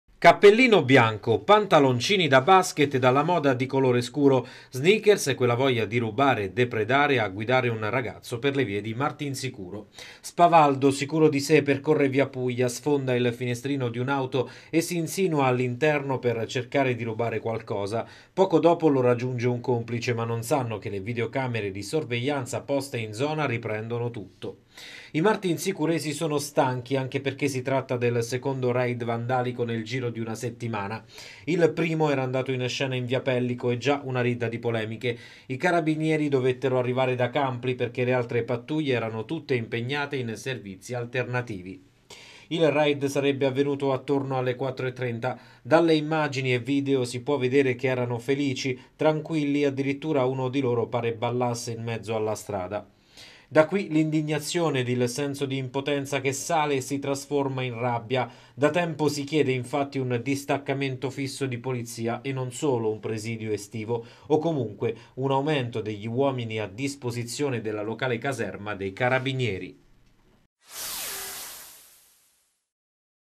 Martinsicuro - Nuovo raid vandalico contro le auto in sosta, colpita via Puglia
Il ragazzo, insieme a un complice, è stato filmato dalle telecamere di videosorveglianza presenti nella zona